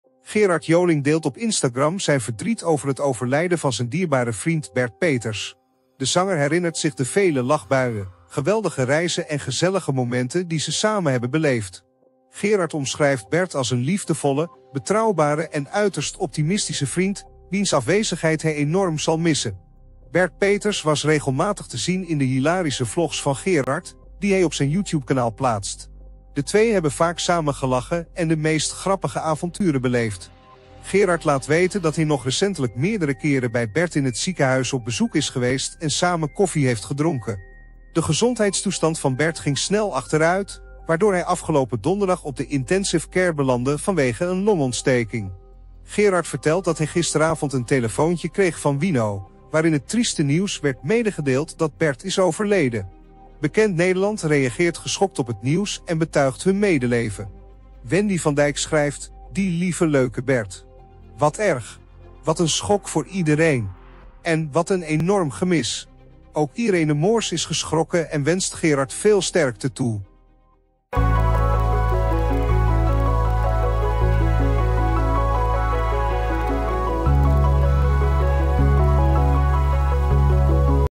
Gerard Joling treurt om het verlies van goede vriend Bert Peters
In deze aangrijpende video deelt Gerard Joling het verdrietige nieuws over het overlijden van zijn goede vriend Bert Peters. Gerard vertelt over de bijzondere band die ze hadden, de hilarische vlogs die ze samen maakten en de mooie momenten die ze deelden. Hij uit zijn diepe rouw en bedankt zijn fans en bekend Nederland voor de steunbetuigingen. Bekijk de video en deel je medeleven in de reacties.